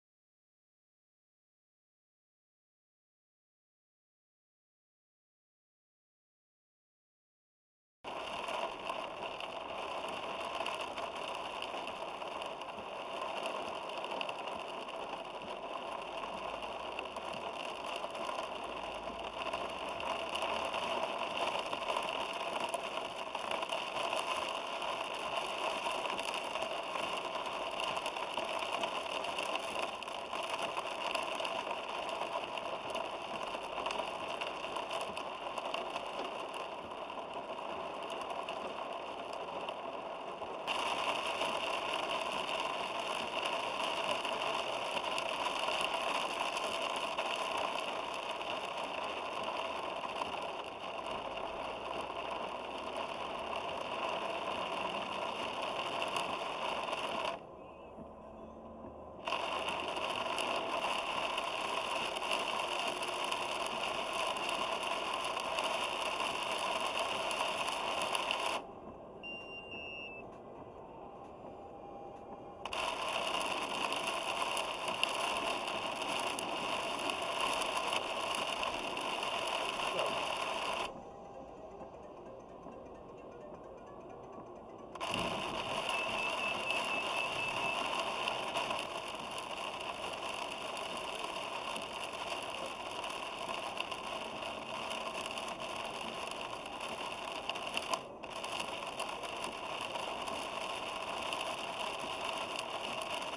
(VIDEO) 13-08-2015 Noodweer barst los boven Brabant
BRABANT - Vanuit Zeeland komen fikse regen, onweer en hagelbuien vergezeld met windstoten onze provincie binnen. Het verkeer in de omgeving van Roosendaal heeft donderdagavond last van zware regenval. Op de snelwegen A17 en A58 kunnen automobilisten soms niet harder rijden dan 30 kilometer per uur, meldt de VerkeersInformatieDienst. Ook op de A16 is het oppassen, de regen komt er met bakken uit de lucht en het zicht is minimaal. De video die hier onderstaat is gemaakt op de A16 bij Breda.

De meldingen van overlast in Bergen op Zoom stromen binnen en ook in andere delen van Brabant is er fikse overlast.